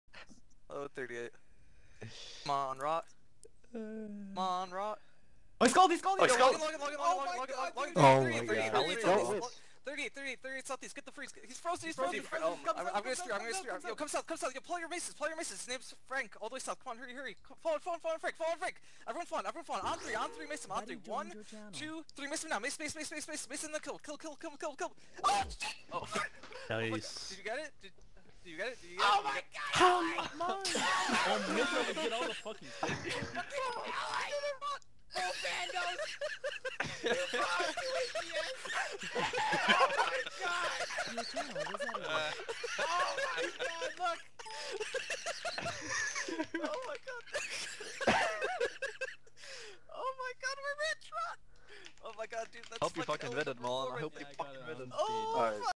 Unsuspecting gamer gets mugged for 600,000,000 gold pieces by RoT
Check our other videos for Compilations of RoT's Lures, Stream Snipes, Single Pking and more!
Comment and tell us how you vote no on pvp polls!

Easy for RoT, stealing an unsuspecting gamers elysian spirit shield.